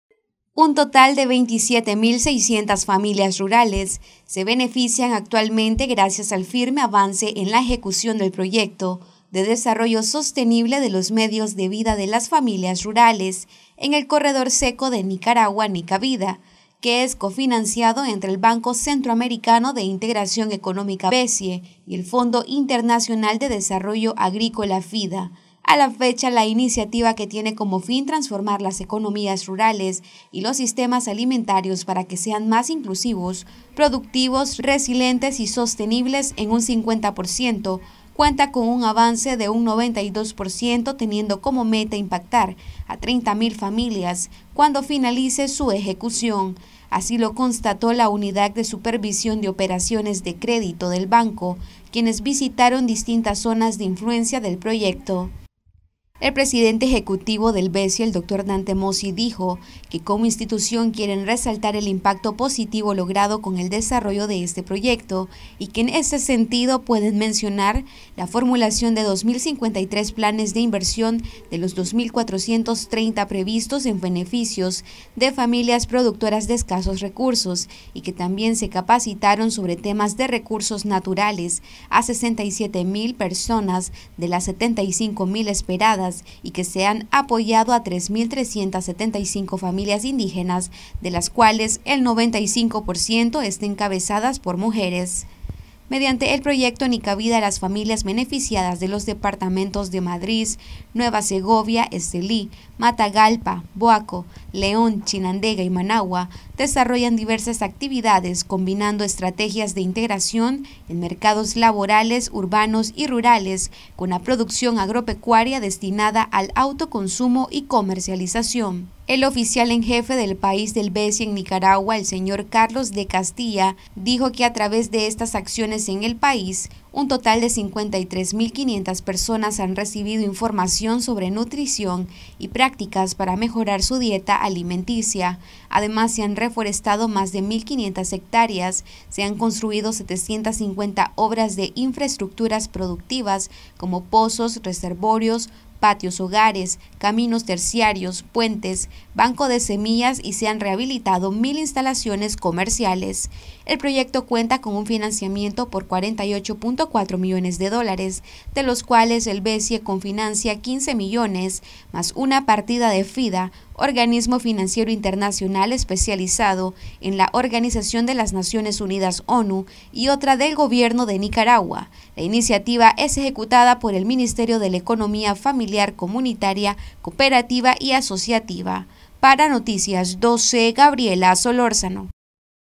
BCIE Impulsa Proyecto de Desarrollo Sostenible para las familias rurales en el Corredor Seco
Noticias12Nicaragua BCIE Impulsa Proyecto de Desarrollo Sostenible para las Familias Rurales en el Corredor Seco de Nicaragua.